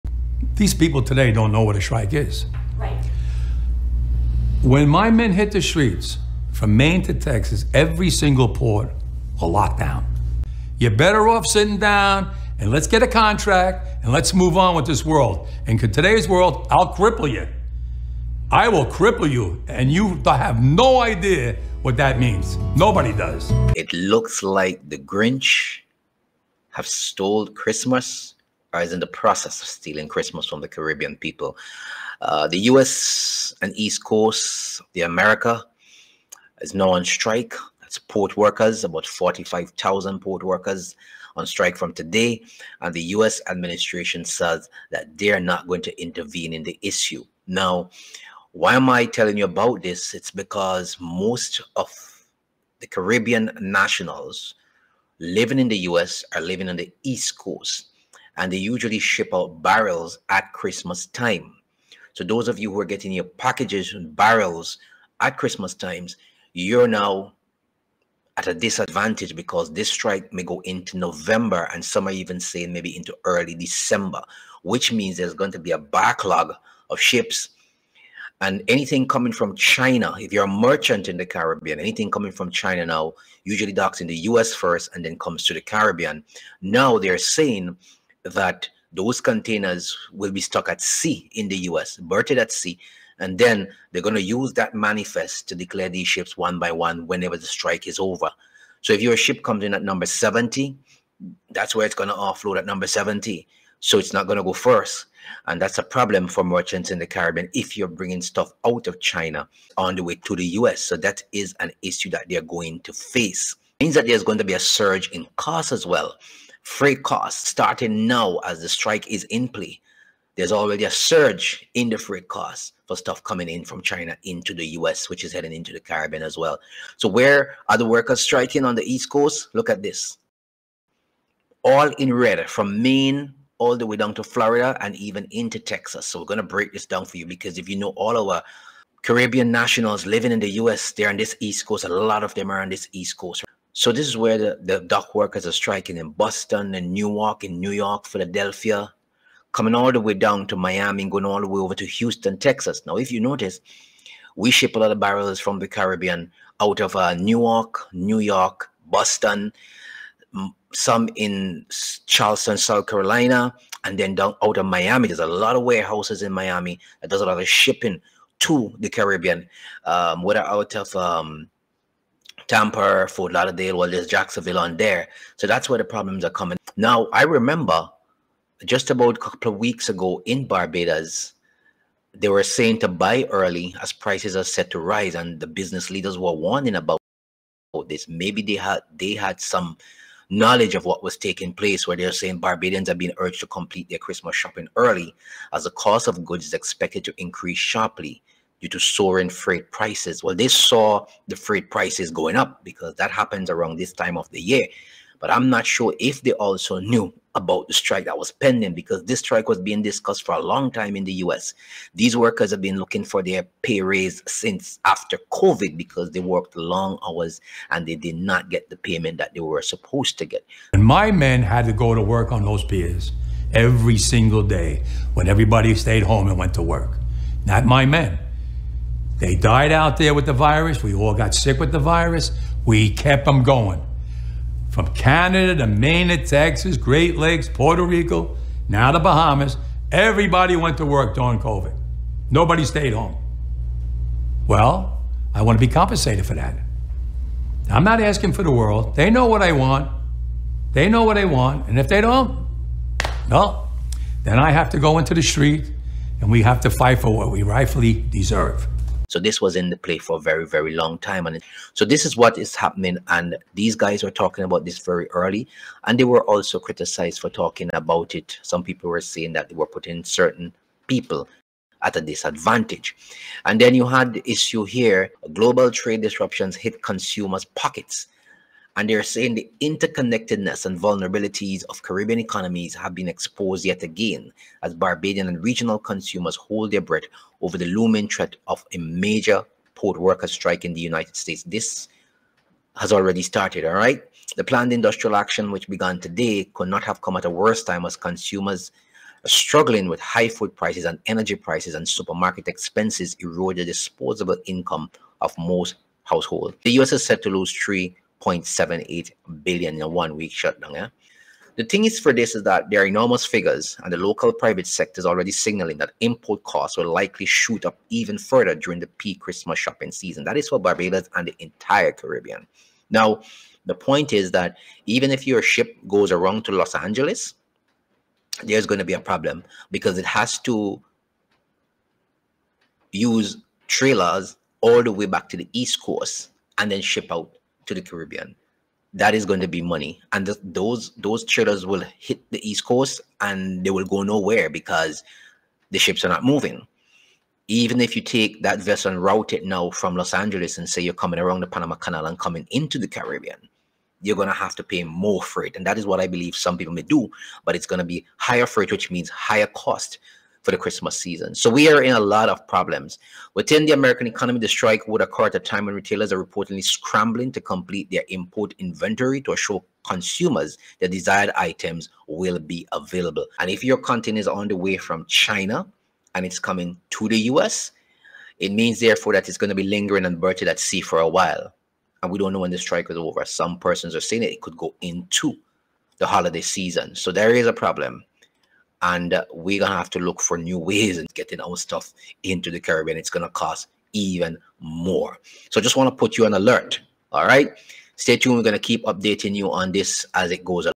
No Barrels For Caribbean People This Christmas? Freight and Food Cost To Skyrocket
The planned industrial action could not have come at a worse time as consumers here struggle with high food and energy prices and supermarket expenses erode the disposable income of most households.